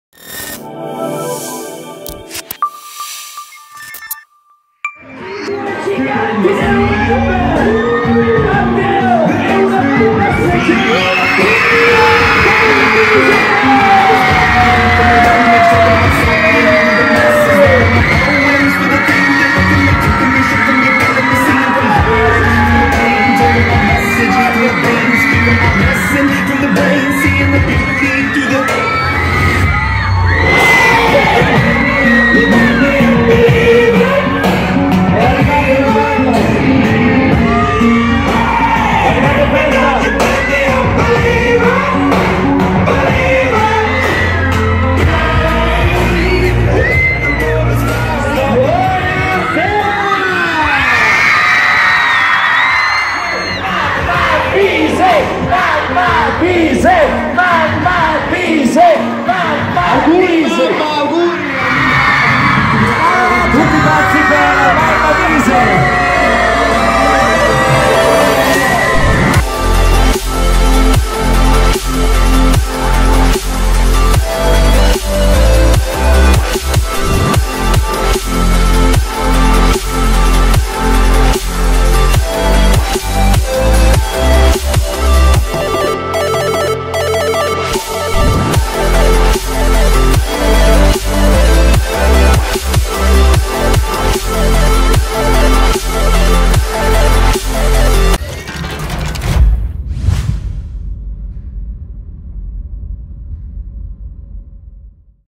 INSTORE DI MATT&BISE AL CC I GIARDINI DEL SOLE 14/05/2017
VIDEO dell'evento con Matt & Bise presso il Centro Commerciale I Giardini del Sole di Castelfranco Veneto (TV) dove incontrano tutti i loro fan ed autografano tutte le copie del loro nuovo LIBRO "UNA MAMMA PER NEMICA"!😍✌